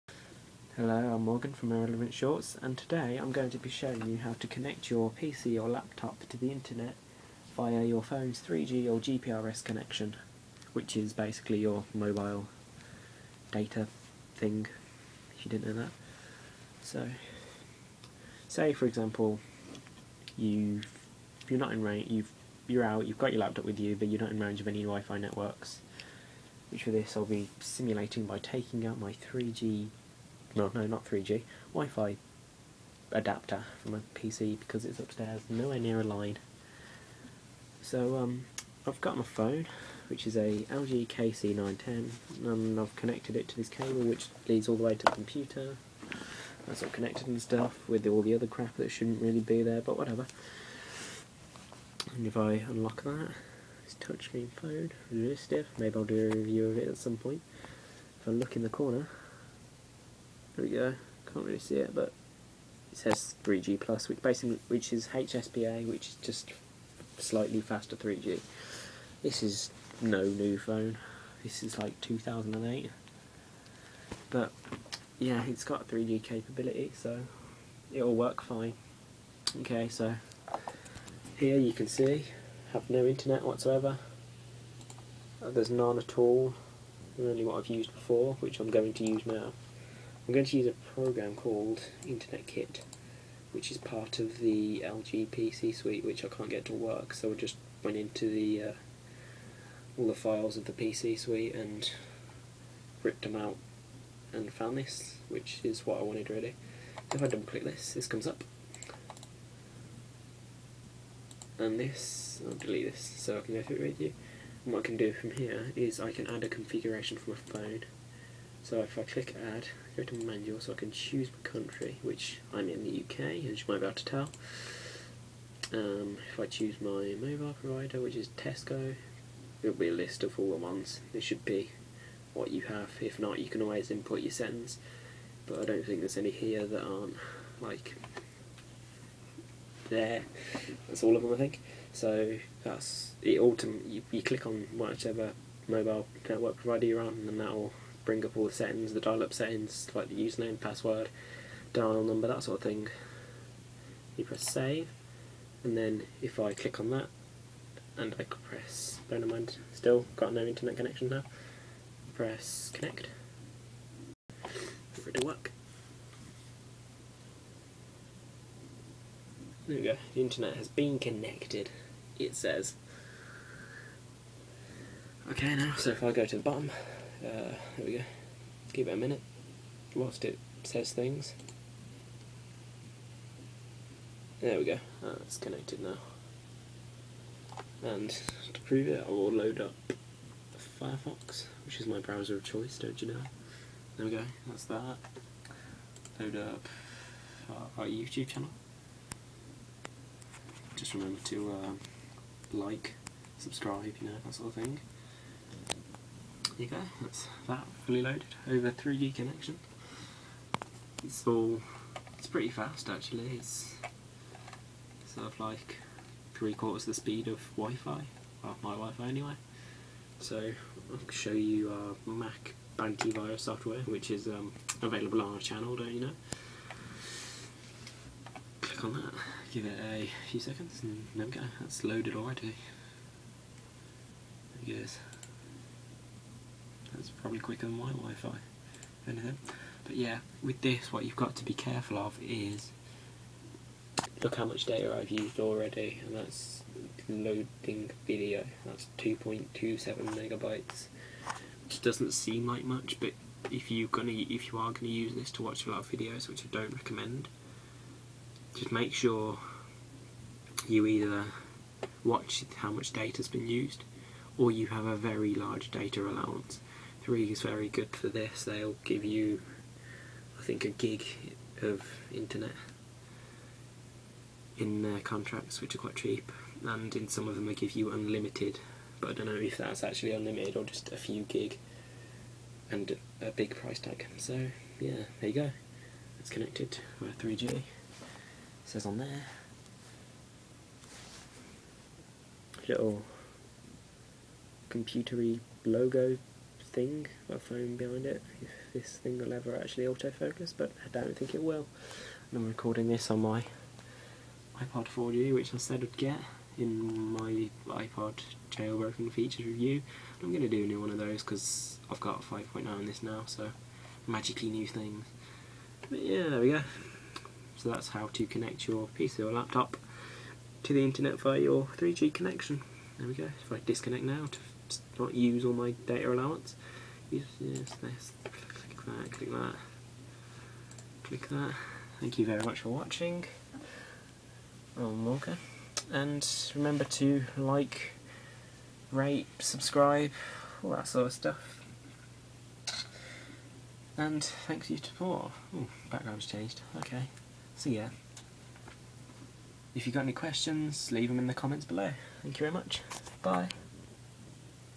How to connect to the internet via USB tethering
Morgan shows you how to connect your pc or laptop to the internet using your phones 3G or GPRS connection. enjoy!